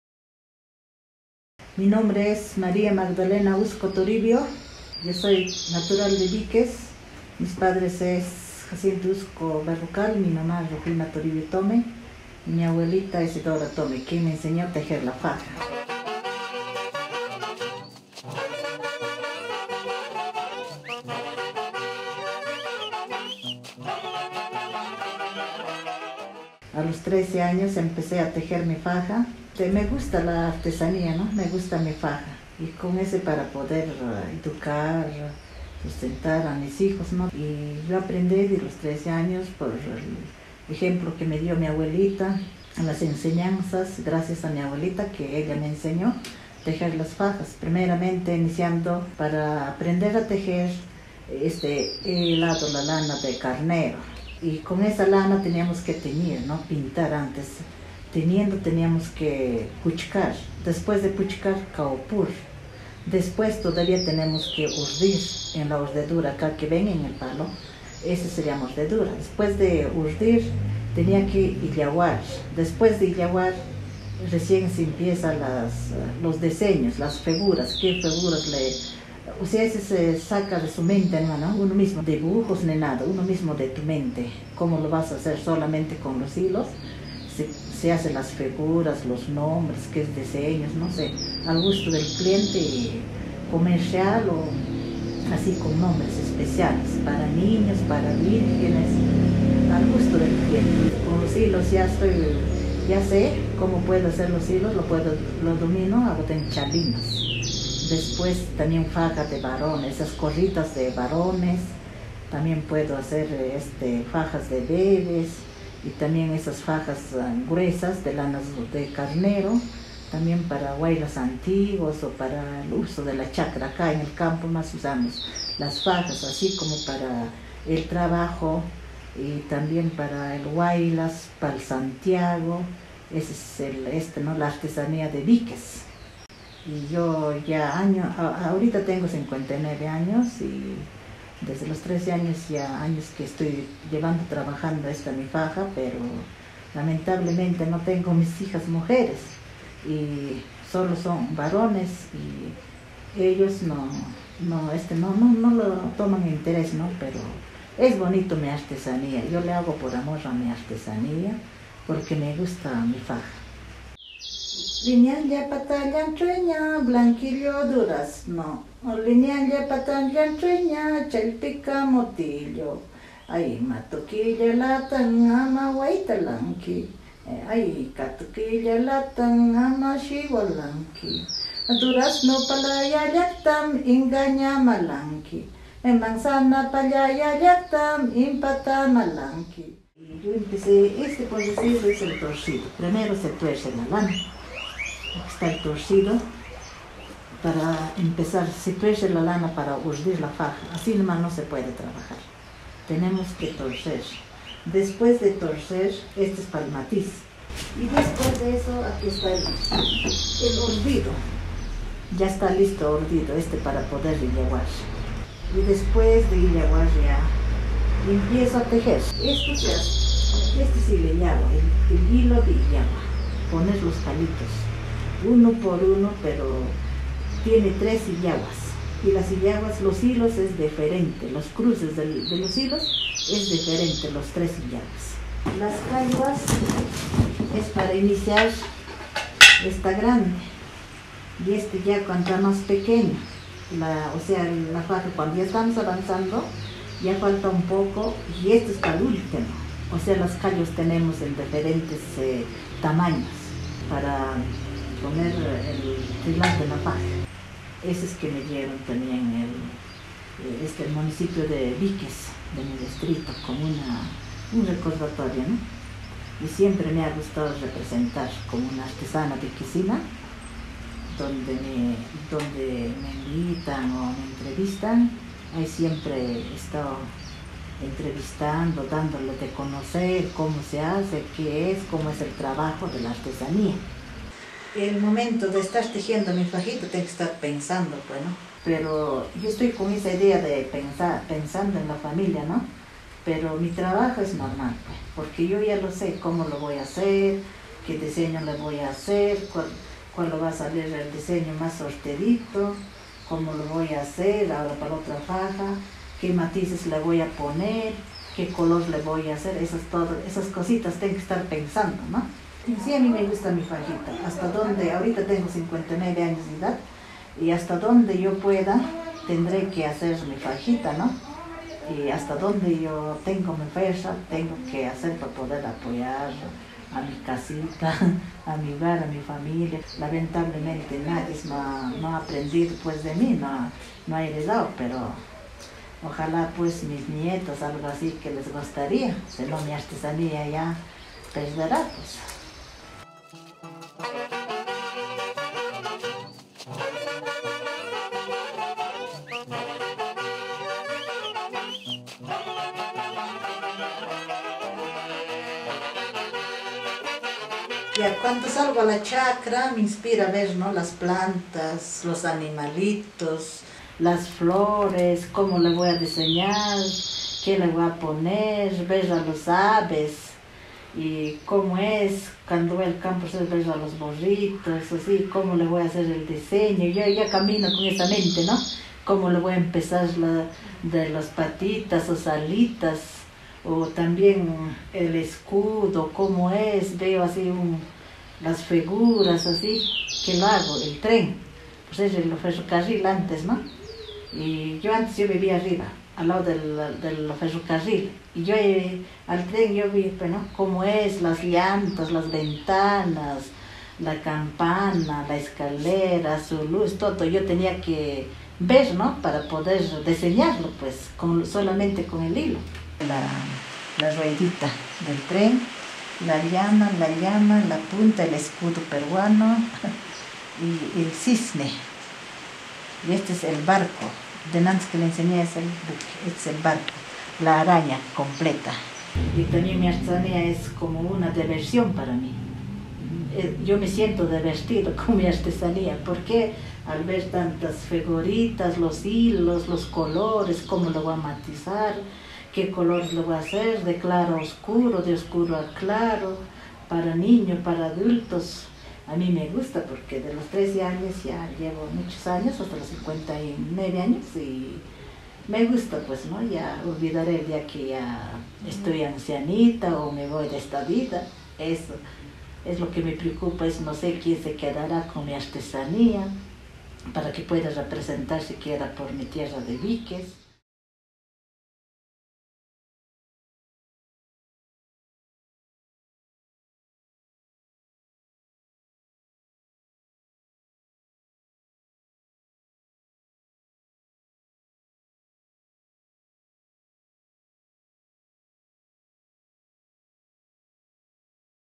María Huzco | La tejedora sin herederas para su arte - HuancaYorkTimes
En el tejido de fajas, los dedos de María Huzco siguen el camino de su imaginación. De ellas nacen finos tejidos. Su arte, sin embargo, no tiene herederas entre los suyos, pero goza del reconocimiento de coleccionistas, investigadores y las sociedad: sus fajas son requeridas para ocasiones de realce como los concursos de huaylarsh y pedidos especiales.